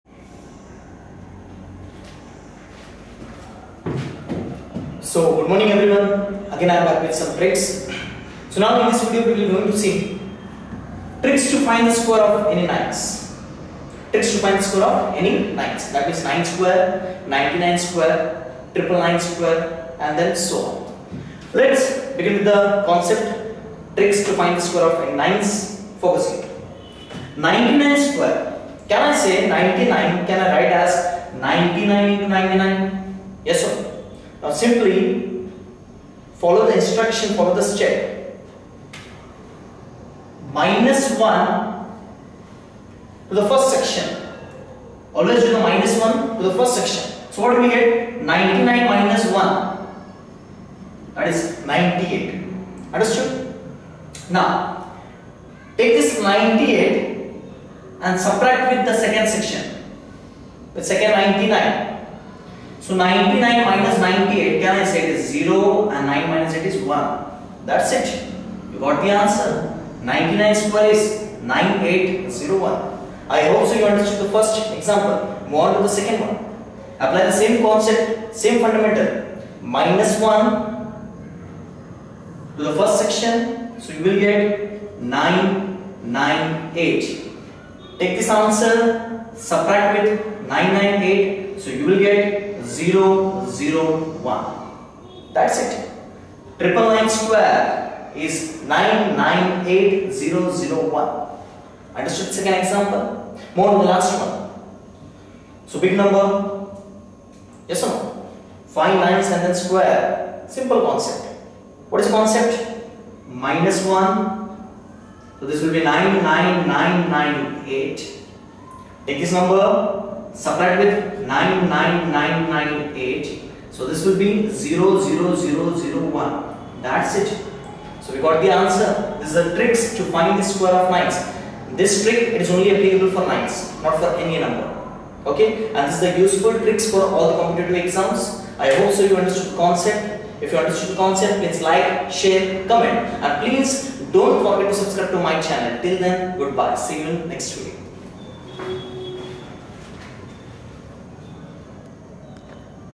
SSC GGL:-TRICKS TO FIND THE SQUARE ,USEFUL TRICKS FOR BANK,SSC CGL...
SHIVA SIR'S
ACHIEVERS ACADEMY
SSC GGL:-TRICKS TO FIND THE SQUARE ,USEFUL TRICKS FOR BANK,SSC CGL...TRICKS TO FIND THE SQUARE FOR 9'S.

SSC CGL:-SHORTCUT TRICK TO FIND SQUARE OF ANY 3 DIGIT NUMBER IN LESS THAN 3 SECOND. (PART-II)